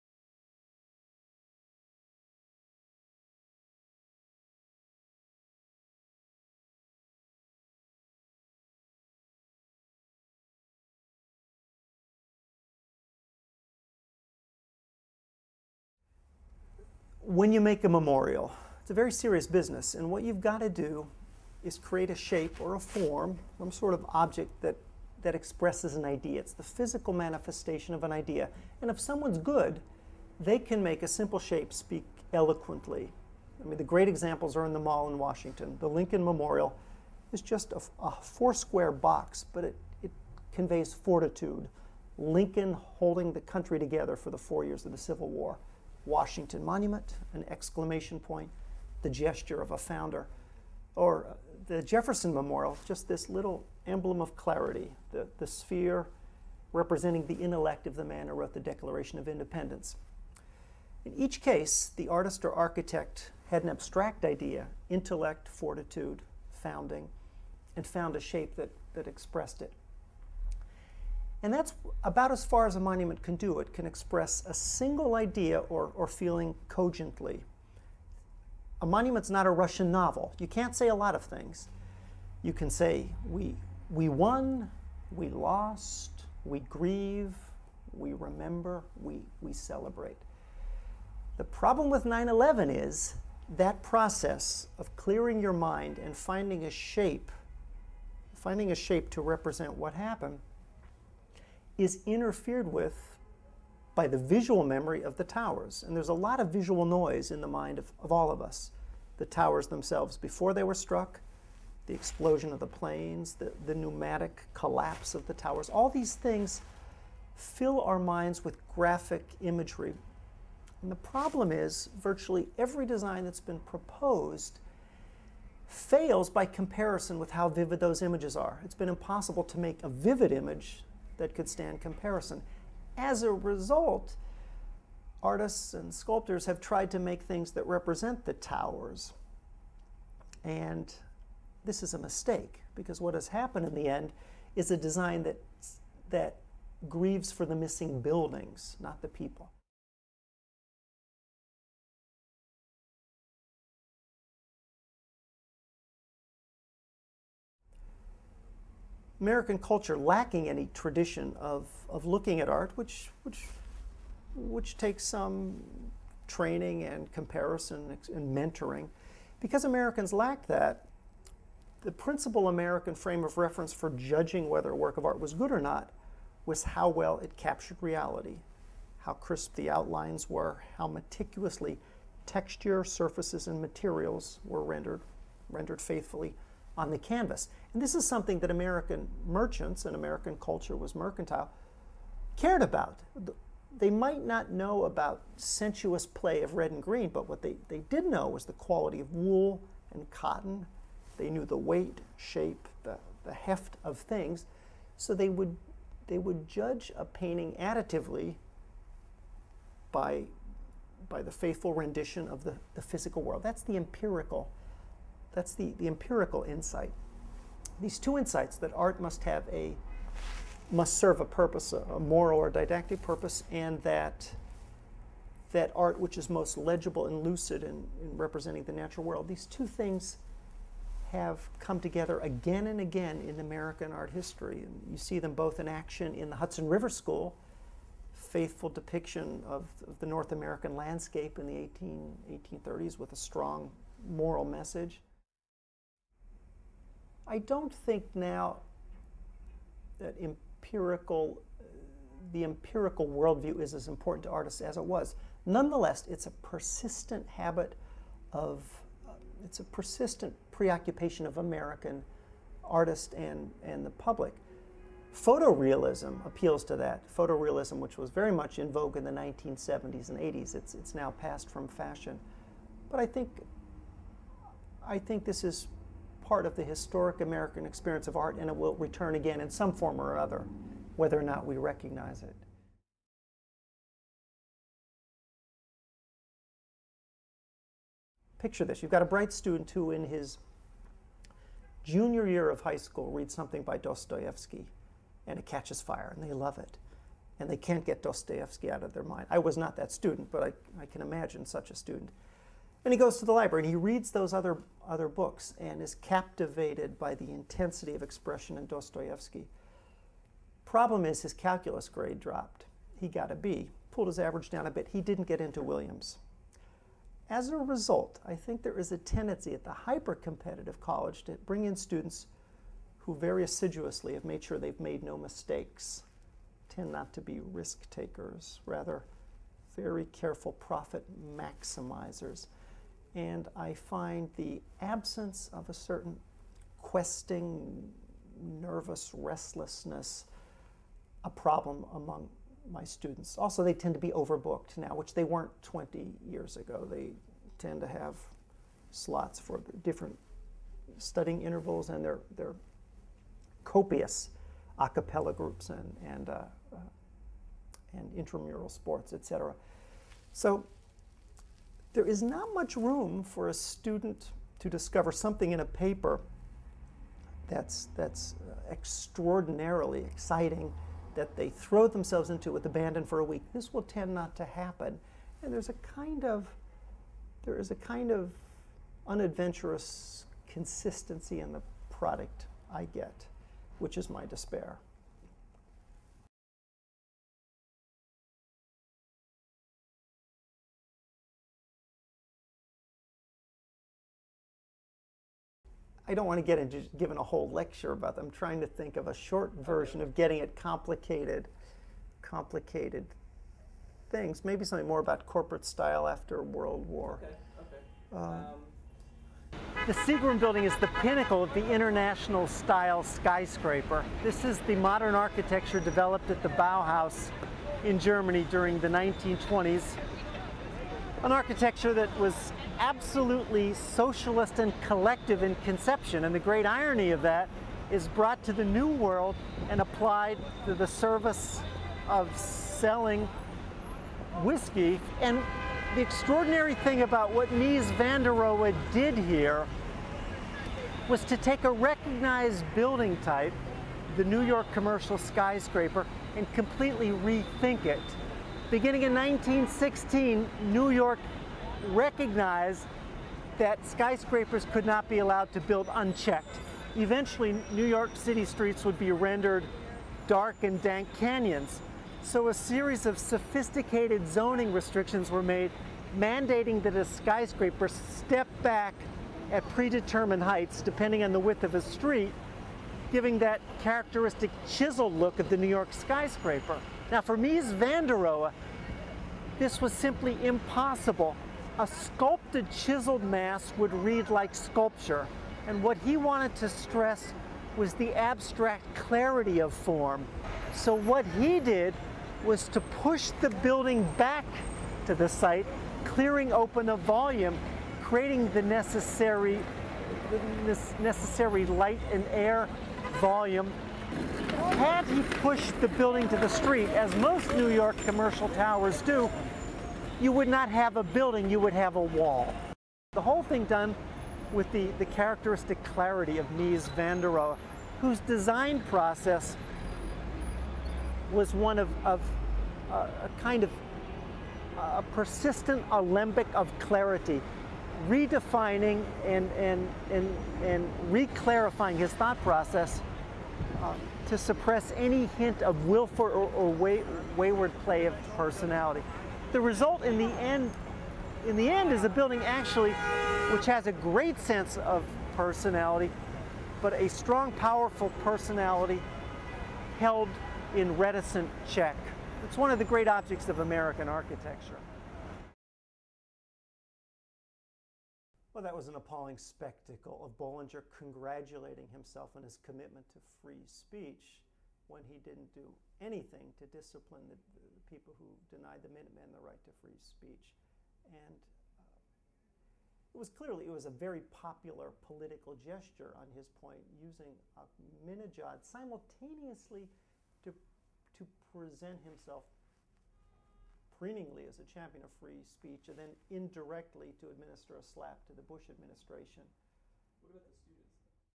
An Interview with Michael J. Lewis
Michael J. Lewis is a professor of art at Williams College and a contributor to the horizon, the arts blog of COMMENTARY. He is the author of American Art and Architecture, a recently published survey of American art history and examination of our nation's distinct architectural heritage. He published "Frank Furness: Architecture and the Violent Mind," a study of the Victorian style on American architects, in 2001, and received the Alice Davis Hitchcock Award in 1994 for The Politics of the German Gothic Revival: August Rechensperger—a critical study of the German architect.

In our interview, Lewis touches on his fascination with the "American empirical" tradition in the arts, the uninspiring and confused proposed designs for a World Trade Center memorial, the bad effects of U.S. News rankings on college students, and the spectacular contribution of Ludwig Mies van der Rohe to American architecture: New York City's Seagram building.